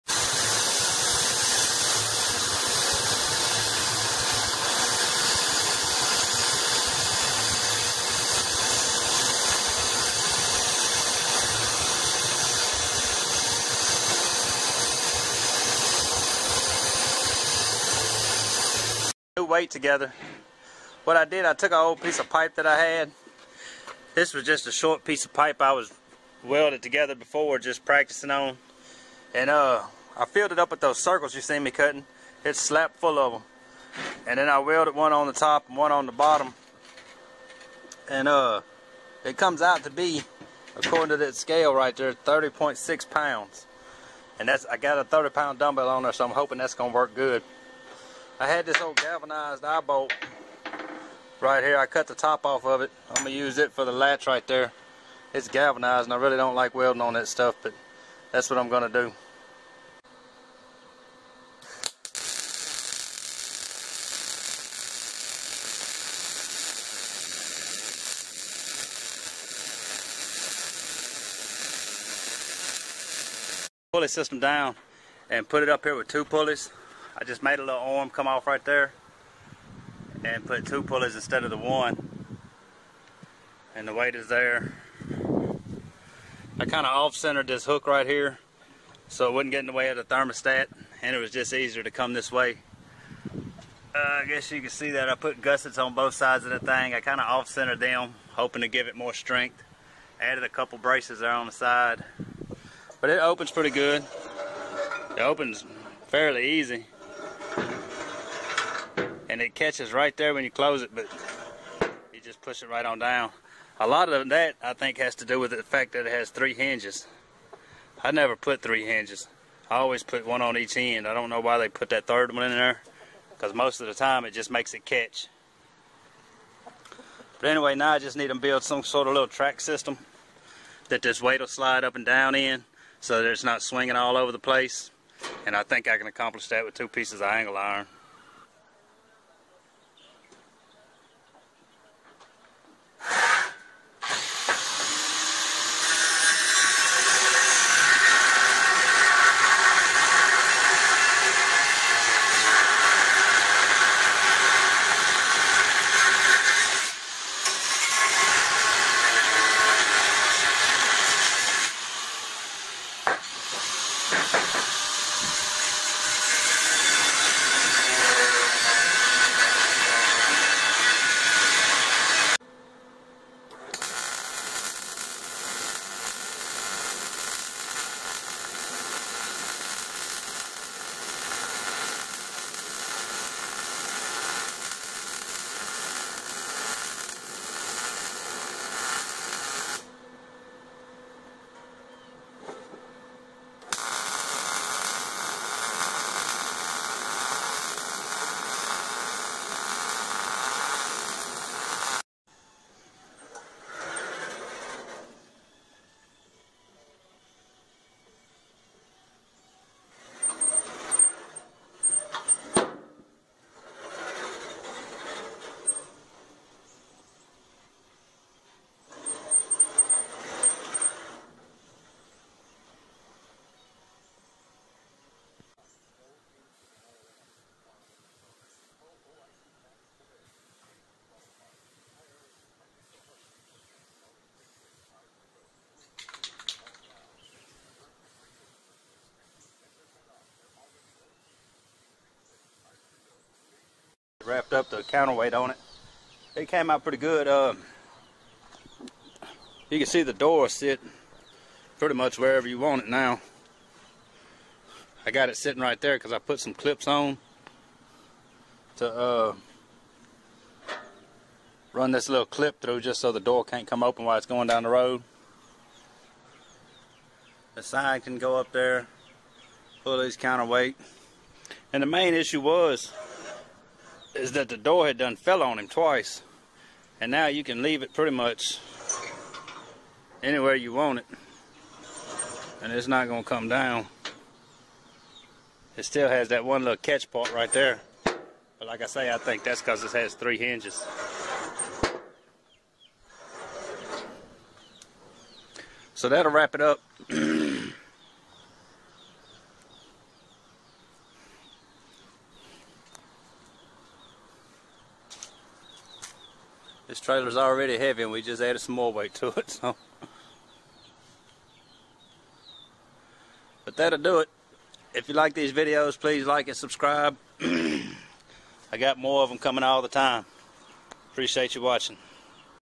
Bbq smoker counter weights build part 2/2
Finishing up the counter weight on this smoker